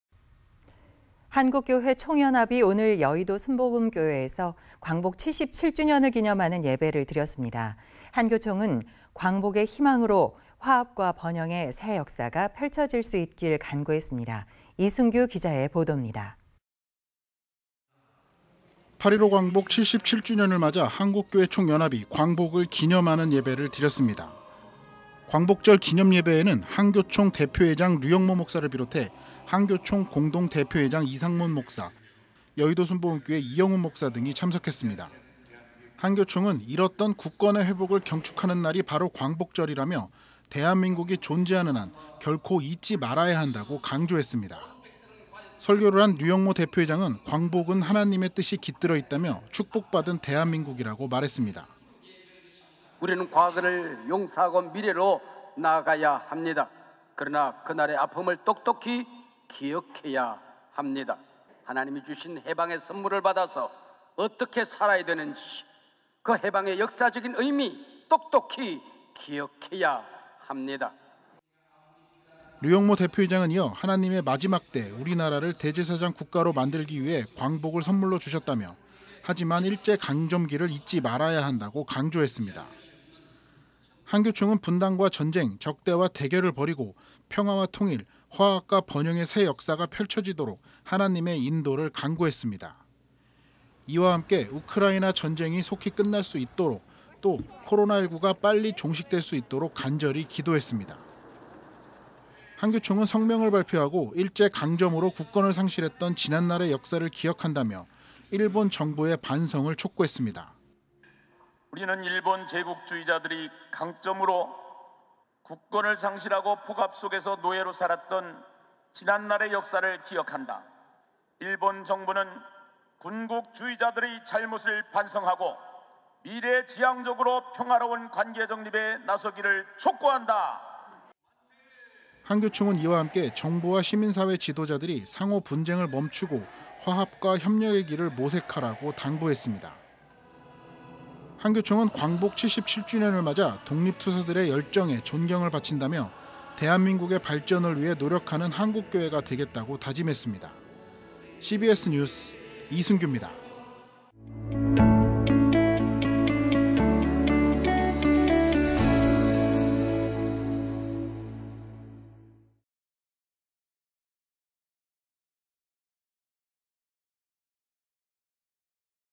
[CBS 뉴스] 한교총, "광복은 하나님의 선물"
한국교회총연합이 10일 여의도순복음교회에서 광복 77주년을 기념하는 예배를 드렸습니다. 한교총은 광복의 희망으로 화합과 번영의 새 역사가 펼쳐질 수 있도록 간구했습니다.

이승규 기자의 보돕니다.

8.15 광복 77주년을 맞아 한국교회총연합이 광복을 기념하는 예배를 드렸습니다. 광복절 기념예배에는 한교총 대표회장 류영모 목사를 비롯해 한교총 공동 대표회장 이상문 목사, 여의도순복음교회 이영훈 목사 등이 참석했습니다. 한교총은 잃었던 국권의 회복을 경축하는 날이 바로 광복절이라며, 대한민국이 존재하는 한 결코 잊지 말아야 한다고 강조했습니다.

설교를 한 류영모 대표회장은 광복은 하나님의 뜻이 깃들어 있다며, 축복 받은 대한민국이라고 말했습니다.

류영모 목사 / 한국교회총연합 대표회장
"우리는 과거를 용서하고 미래로 나아가야 합니다. 그러나 그날의 아픔을 똑똑히 기억해야 합니다. 하나님이 주신 해방의 선물을 받아서 어떻게 살아야 되는지 그 해방의 역사적 의미 똑똑히 기억해야 합니다."

류영모 대표회장은 이어 하나님이 마지막 때 우리나라를 대제사장 국가로 만들기 위해 광복을 선물로 주셨다며, 하지만 일제 강점기를 잊지 말아야 한다고 강조했습니다.

한교총은 분단과 전쟁, 적대와 대결을 버리고 평화와 통일, 화합과 번영의 새 역사가 펼쳐지도록 하나님의 인도를 간구했습니다. 이와 함께 우크라이나 전쟁이 속히 끝날 수 있도록 또 코로나 19가 빨리 종식될 수 있도록 간절히 기도했습니다.

한교총은 또 성명을 발표하고 일제 강점으로 국권을 상실했던 지난날의 역사를 기억한다며, 일본 정부의 반성을 촉구했습니다.

강학근 목사·김기남 목사 / 한국교회총연합 공동 대표회장
"우리는 일본 제국주의자들의 강점으로 국권을 상실하고 폭압 속에서 노예로 살았던 지난날의 역사를 기억한다. 일본 정부는 군국주의자들의 잘못을 반성하고, 미래지향적으로 평화로운 관계 정립에 나서기를 촉구한다."

한교총은 이와 함께 정부와 시민사회 지도자들이 상호분쟁을 멈추고 화합과 협력의 길을 모색하라고 당부했습니다.

한교총은 광복 77주년을 맞아 독립투사들의 열정에 존경을 바친다며, 대한민국의 발전을 위해 노력하는 한국교회가 되겠다고 다짐했습니다.

CBS 뉴스 이승규입니다.